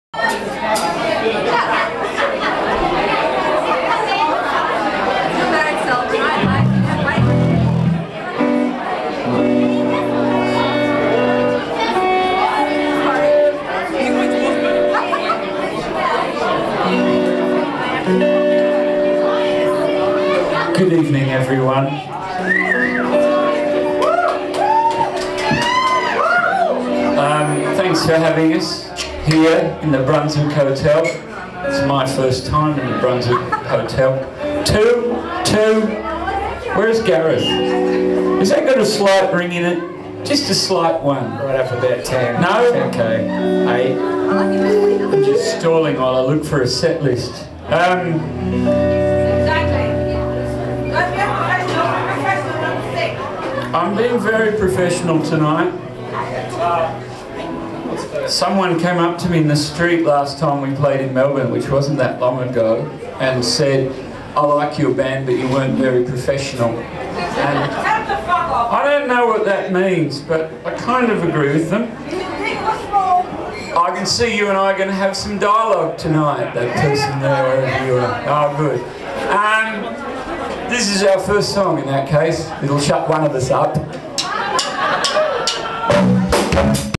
Pinky on being professional - Brunswick Hotel 25 July 2008
Opening greeting and comment on being professional. Brunswick Hotel 25 July 2008.